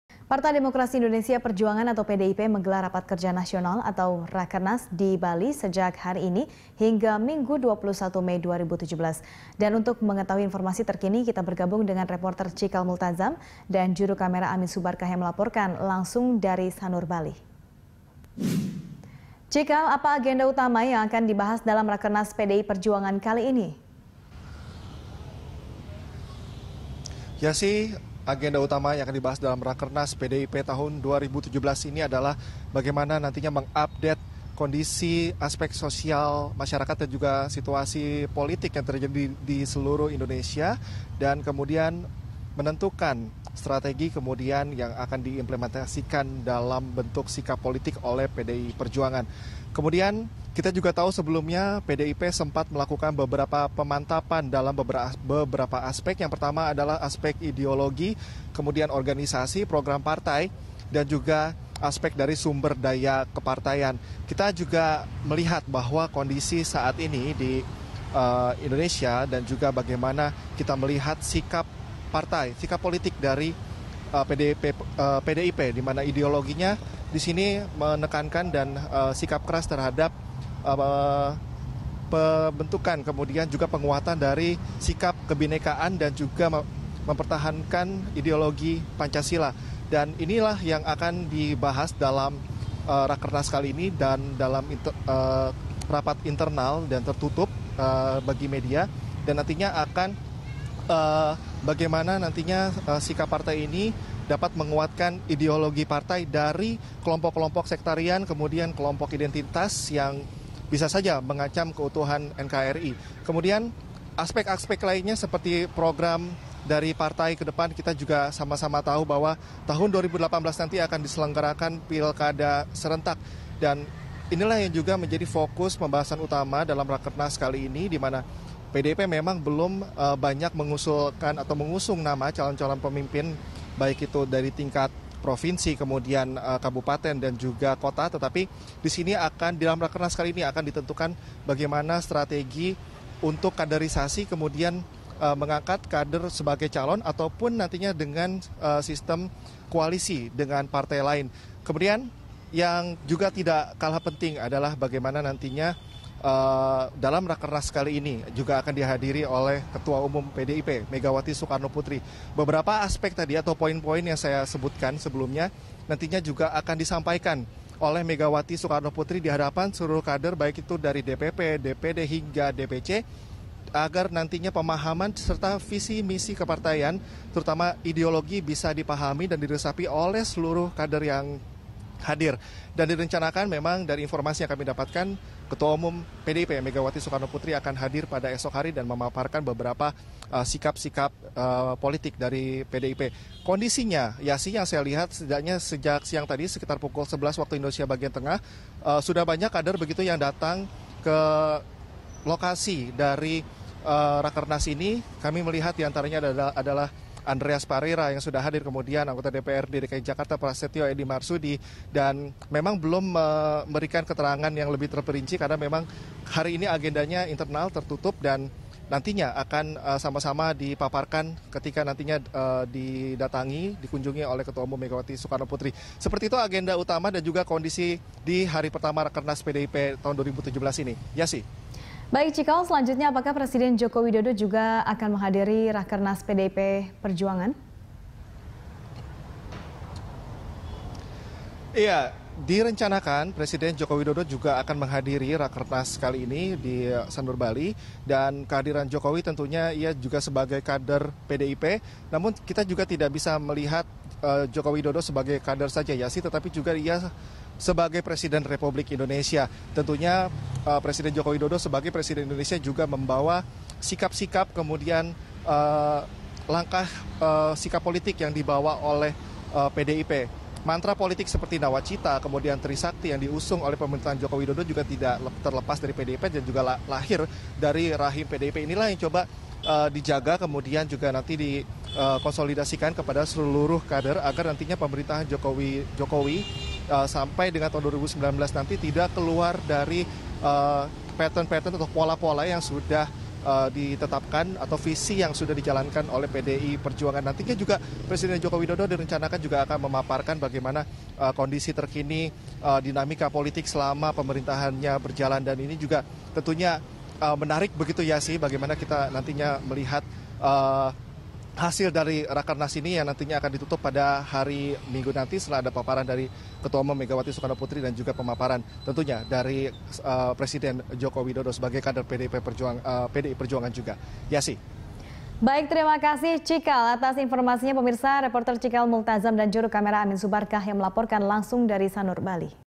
PDI Perjuangan Gelar Rakernas di Bali
Partai Demokrasi Indonesia Perjuangan atau PDI Perjuangan menggelar Rapat Kerja Nasional di Bali hingga Minggu (21/05) besok.

Facebook.com/BeritaSatuTV
Youtube.com/BeritaSatu